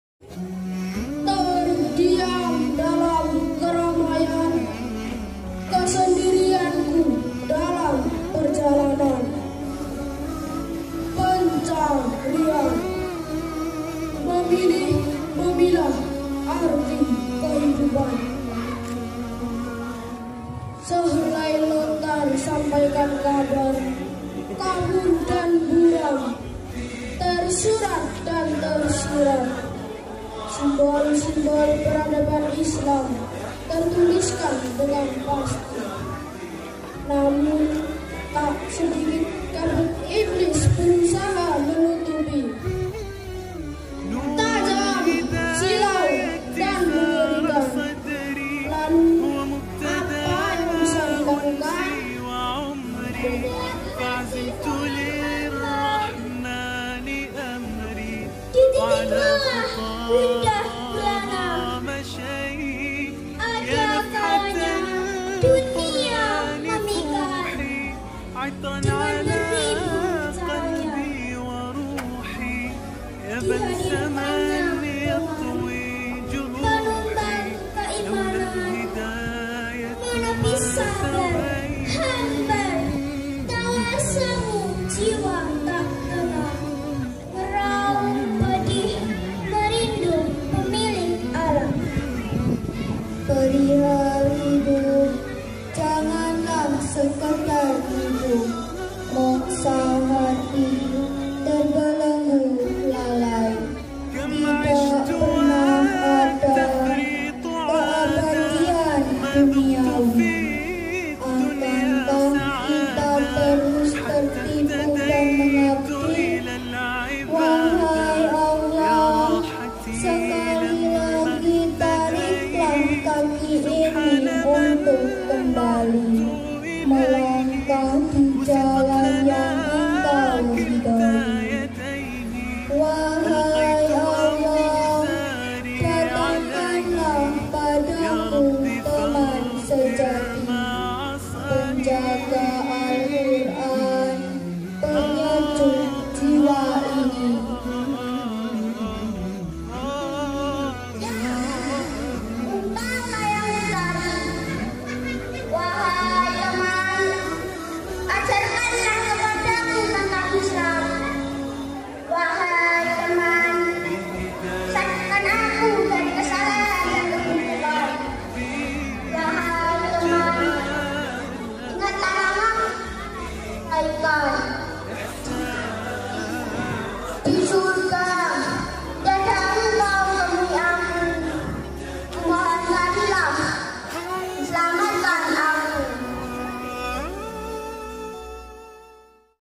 PUISI KOLOSAL " TEMAN SEJATI" Perwakilan kelas 3 | Haflah Akhirussanah| SDIT Al Wakil
TEMAN SEJATI
(Karya "Endang Wulandari")

Terdiam dalam keramaian
Kesendirian ku dalam perjalanan
Pencarian,
Memilih memilah arti kehidupan

Sehelai lontar sampaikan kabar,
Kabur dan buram
Tersurat dan tersirat,
Simbol-simbol peradaban Islam tertuliskan dengan pasti
Namun tak sedikit kabut iblis berusaha menutupi
Tajam, silau dan mengerikan
Lalu apa yang bisa dilakukan?

Gejolak hati seorang hamba
Dititik lelah gundah gulana
Ada kalanya
Dunia memikat,
dengan beribu caranya
Dihadirkannya teman,
penumbang keimanan
Menepis sabar
Hambar, tawa semu jiwa tak tenang
Meraung pedih merindu pemilik alam

Perihal hidup janganlah sekedar hidup
Moksa hati terbelenggu lalai
Tidak pernah ada, keabadian duniawi
Akankah kita terus tertipu dan mengabdi ?

Wahai Allah
Sekali lagi tariklah kaki ini untuk kembali,
Melangkah dijalan yang Engkau ridhoi

Wahai Allah
Datangkanlah pada ku teman sejati
Penjaga Al Qur'an penyejuk jiwa ini

Ya... engkaulah yang kucari...
Wahai teman...
Ajarkanlah kepada ku tentang Islam
Wahai teman...
Sadarkan aku,
dari kesalahan dan keburukan
Wahai teman...
Ingatlah namaku dalam kebaikan
Jika nanti...
Disurga, tiada engkau temuai aku
Kumohon carilah, dan selamatkan aku